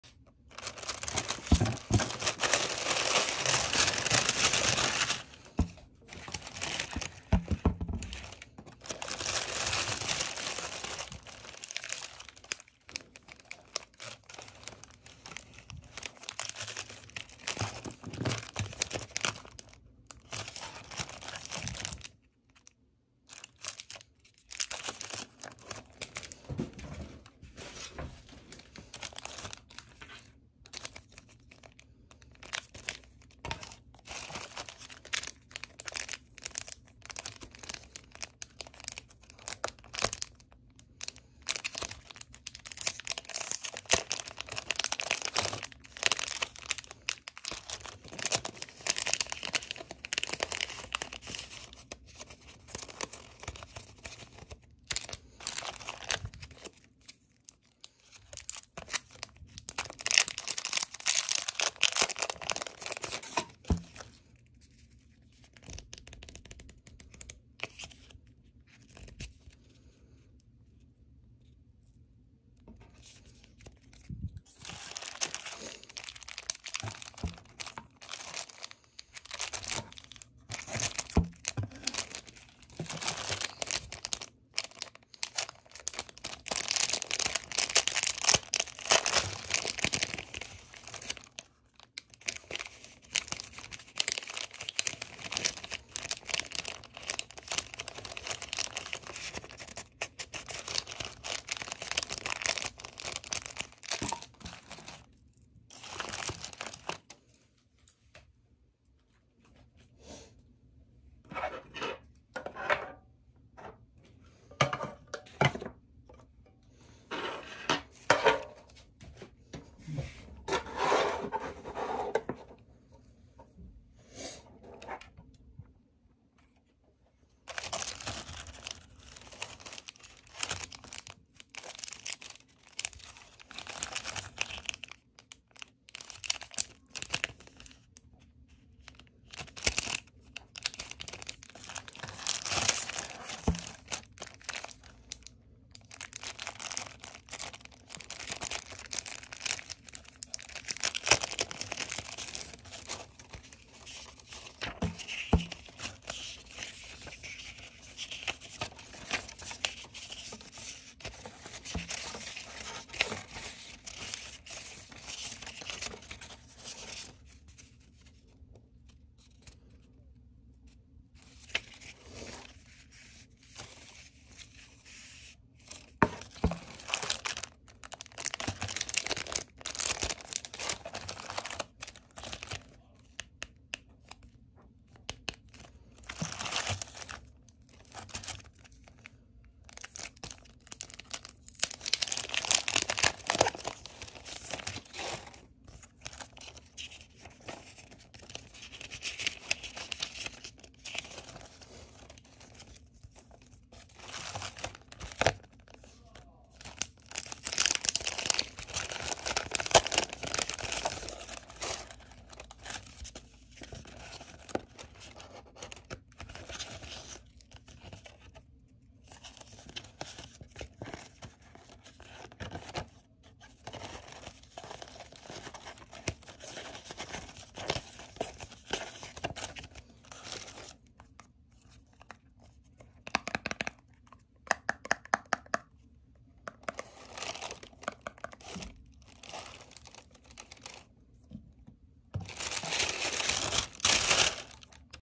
🎁 다꾸 랜덤박스 산타월드
오늘은 다꾸재료 랜덤박스가 도착했습니다(˵ •̀ ᴗ - ˵ )
빈티지 주제로 테마를 골라봤어요!
저는 마음에 쏙드는데 여러분은 어떠신가요?ㅎㅎ

다꾸영상 꾸준히 업로드 예정입니다.
빈틈없이 꽉꽉 채우는 게 뿌듯해요.